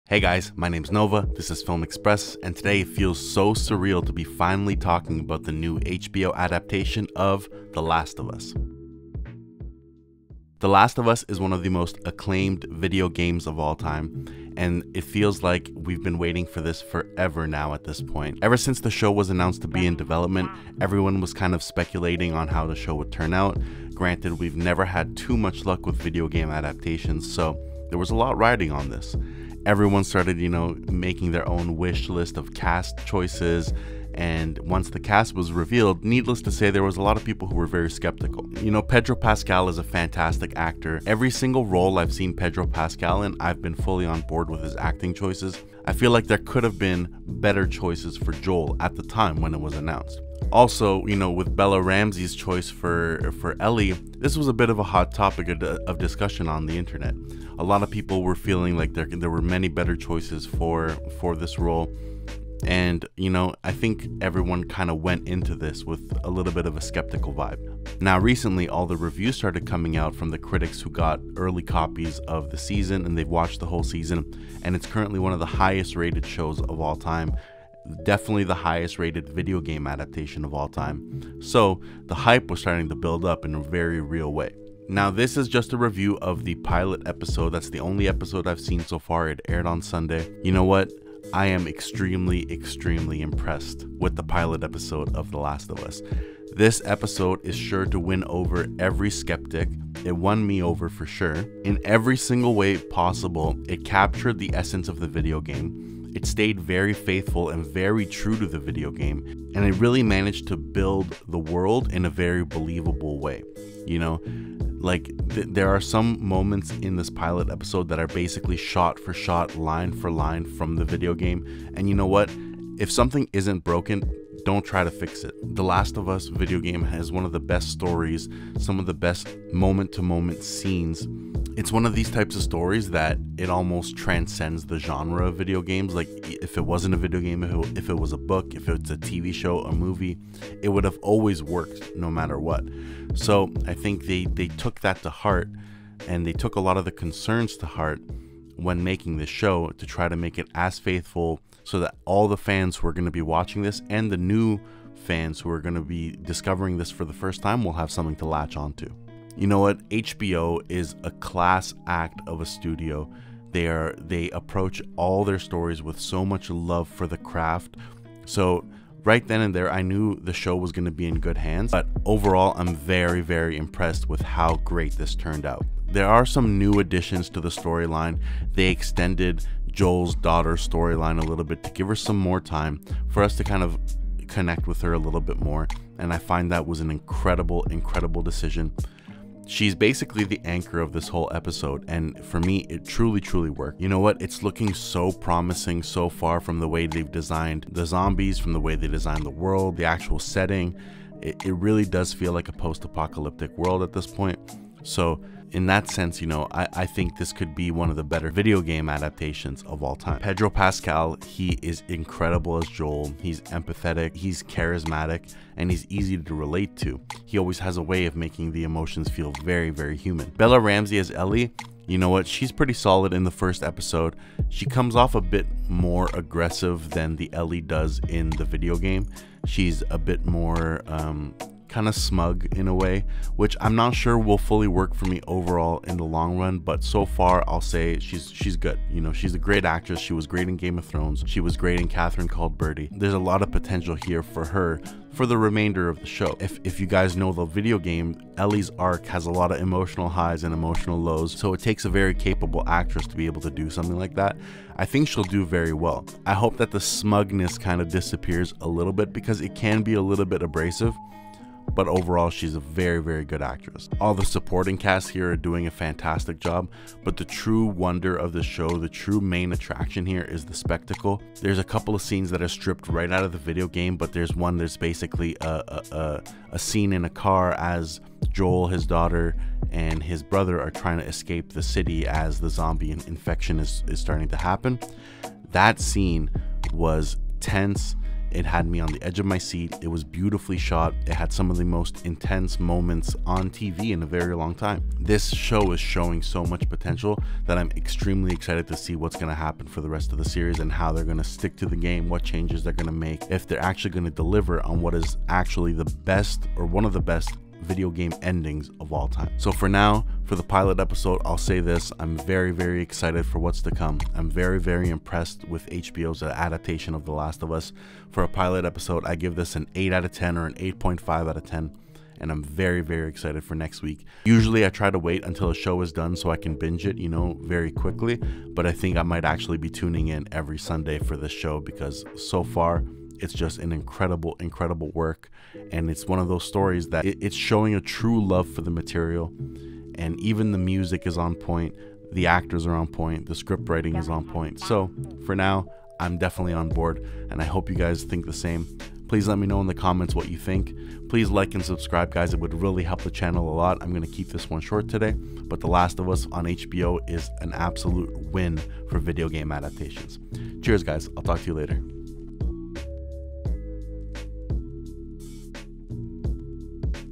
The Last of Us (HBO) S1E1 Review - Very Promising
The long awaited release of THE LAST OF US on HBO is finally here!
After seeing all the hype reviews, claiming this is one of the best video game adaptations ever made, I just had to see this for myself.

I must say, I'm incredibly impressed with HBO's faithful adaptation. They're showing true respect to the fans and a love for the game. The characters, the writing, the spectacle and the world building are all pristine. This pilot episode is VERY MUCH worth watching and I'll be tuning in for the rest of the series!

So if you're a fan of the game or just looking for a new series to watch, then I would highly recommend checking out The Last of Us (HBO) pilot. It's definitely one of the most promising new shows to come out in a while!

8.25/10 !!!

This is Film Express!